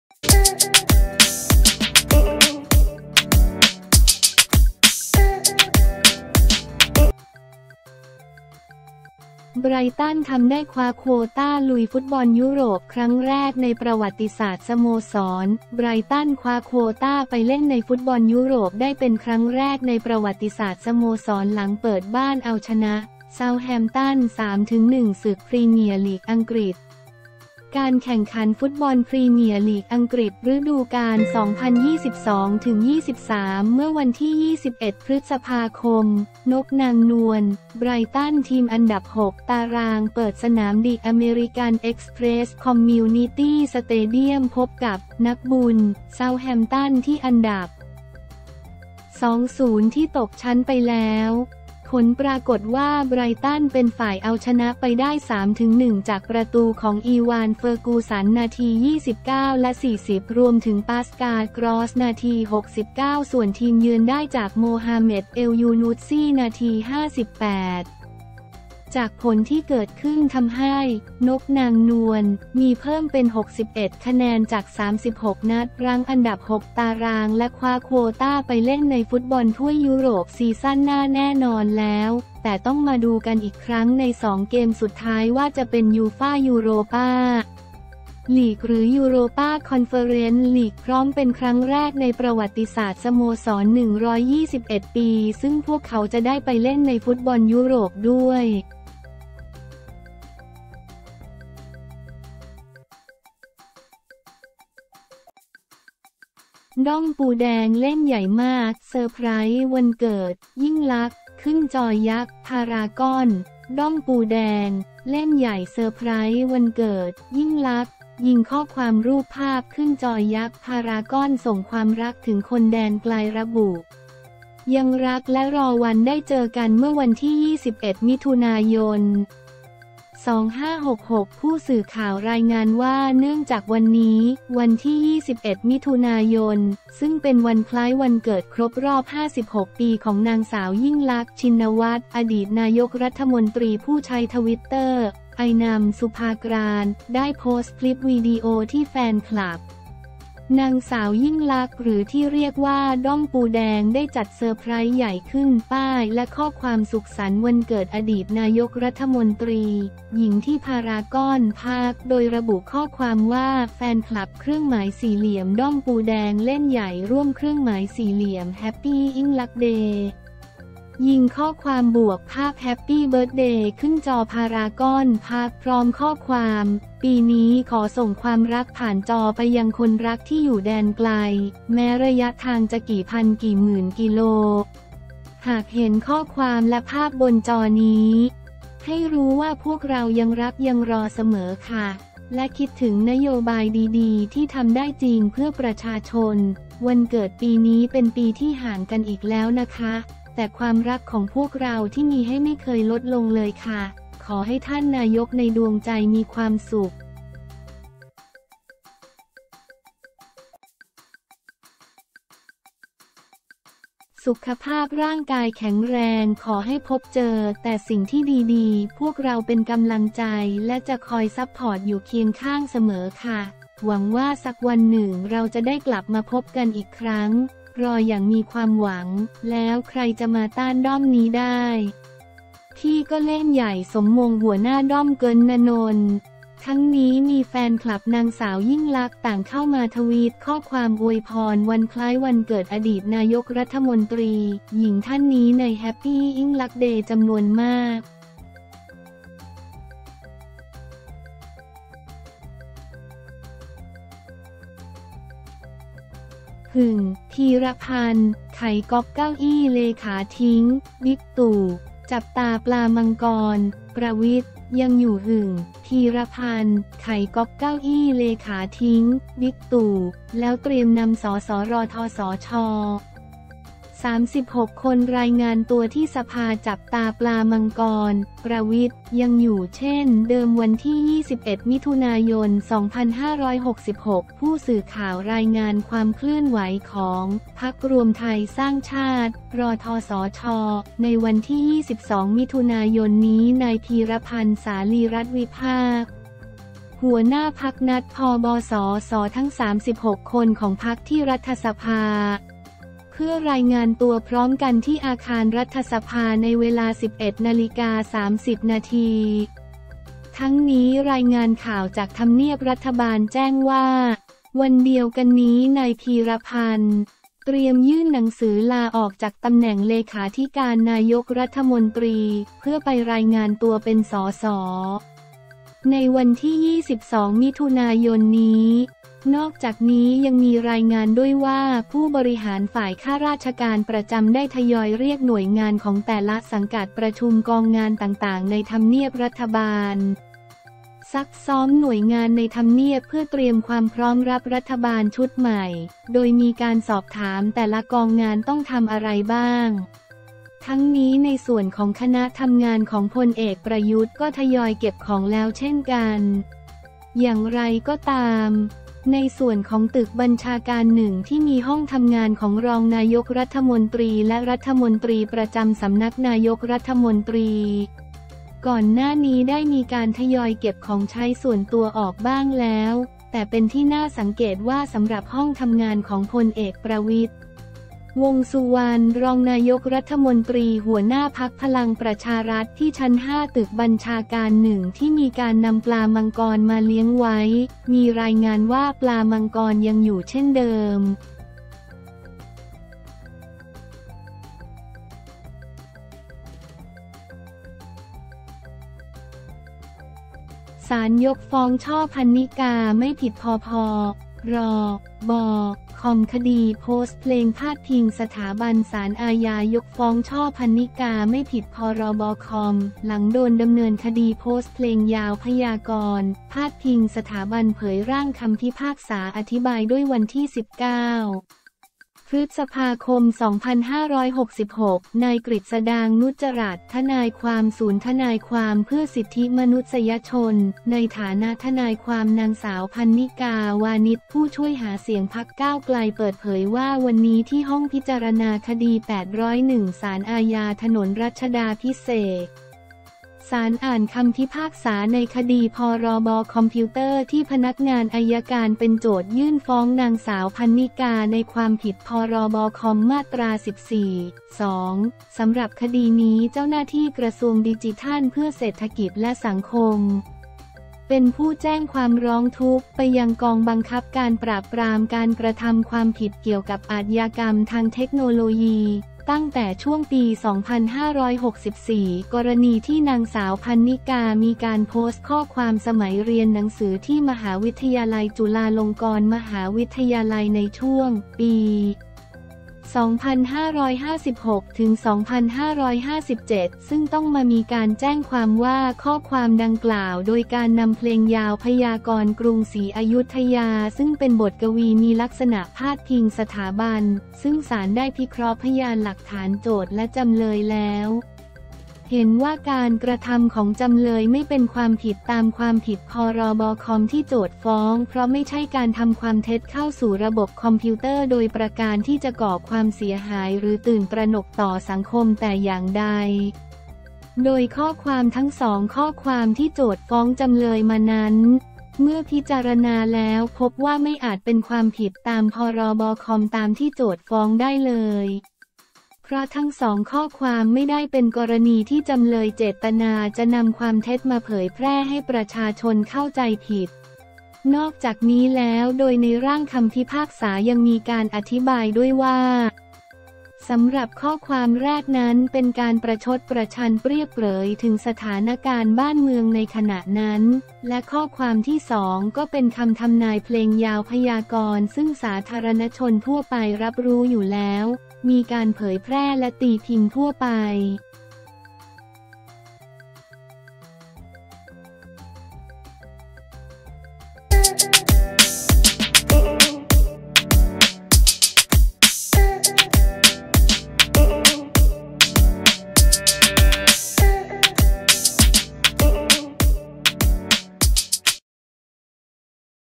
ไบรตัน ทำได้คว้าโควตาลุยฟุตบอลยุโรปครั้งแรกในประวัติศาสตร์สโมสร
ไบรตัน คว้าโควตาไปเล่นในฟุตบอลยุโรปได้เป็นครั้งแรกในประวัติศาสตร์สโมสรหลังเปิดบ้านเอาชนะ เซาธ์แฮมป์ตัน 3-1 ศึกพรีเมียร์ลีก อังกฤษ

การแข่งขันฟุตบอลพรีเมียร์ลีก อังกฤษ ฤดูกาล 2022-23 เมื่อวันที่ 21 พ.ค.”นกนางนวล” ไบรตัน ทีมอันดับ 6 ตาราง เปิดสนาม ดิ อเมริกัน เอ็กซ์เพรสส์ คอมมิวนิตี้ สเตเดียม พบกับ “นักบุญ” เซาธ์แฮมป์ตัน ที่อันดับ 20 ที่ตกชั้นไปแล้ว
ผลปรากฏว่า ไบรตัน เป็นฝ่ายเอาชนะไปได้ 3-1 จากประตูของ อีวาน เฟอร์กูสัน นาที 29 และ40 รวมถึงปาสกาล กรอสส์ นาที 69 ส่วนทีมเยือนได้จาก โมฮาเหม็ด เอลยูนูสซี นาที 58

จากผลที่เกิดขึ้นทำให้ “นกนางนวล” มีเพิ่มเป็น 61 คะแนนจาก 36 นัดรั้งอันดับ 6 ตาราง และคว้าโควตาไปเล่นในฟุตบอลถ้วยยุโรปซีซั่นหน้าแน่นอนแล้ว แต่ต้องมาดูกันอีกครั้งใน 2 เกมสุดท้ายว่าจะเป็น ยูฟ่า ยูโรปา ลีก หรือยูโรปา คอนเฟอเรนซ์ ลีก พร้อมเป็นครั้งแรกในประวัติศาสตร์สโมสร 121 ปีซึ่งพวกเขาจะได้ไปเล่นในฟุตบอลยุโรปด้วย